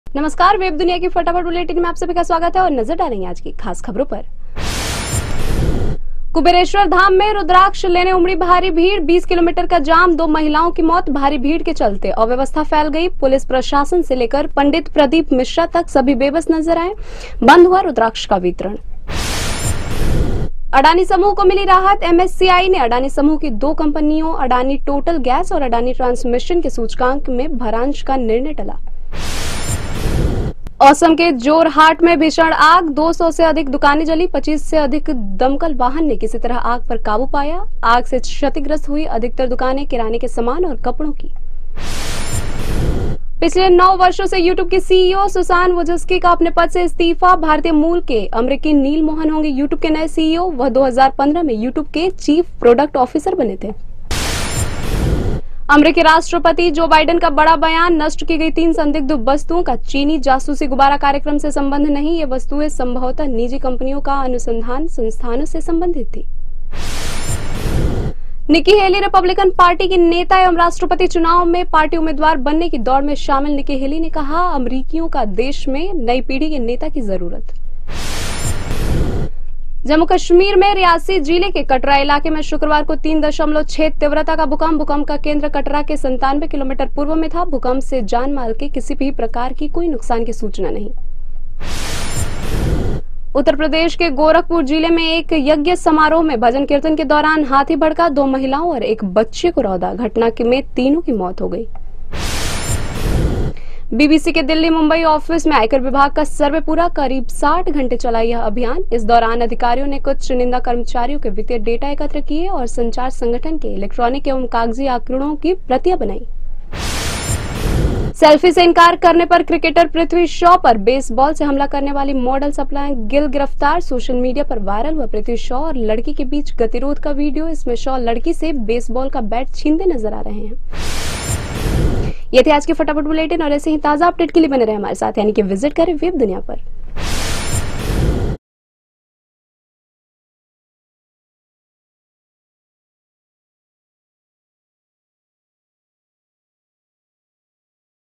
पंडित प्रदीप मिश्रा के कुबेरेश्वर धाम में रुद्राक्ष लेने पहुंचे लाखों लोग, 20 किलोमीटर लंबा जाम
कुबेरेश्वर धाम में रुद्राक्ष लेने उमड़ी भारी भीड़... 20 किलोमीटर का जाम... 2 महिलाओं की मौत...  भारी भीड़ के चलते अव्यवस्था फैल गई... पुलिस, प्रशासन से लेकर पंडित प्रदीप मिश्रा तक सभी बेबस नजर आए... बंद हुआ रुद्राक्ष का वितरण...

कुबेरेश्वर धाम में रुद्राक्ष महोत्सव का 16 फरवरी से 22 फरवरी तक चलना है लेकिन लोगों की भारी भीड़ को देखते हुए महोत्सव को 14 फरवरी से ही शुरू कर दिया गया। लाखों की संख्या में एक साथ लोगों के आने से भोपाल-इंदौर हाइवे पर कई किलोमीटर लंबा जाम भी लग गया।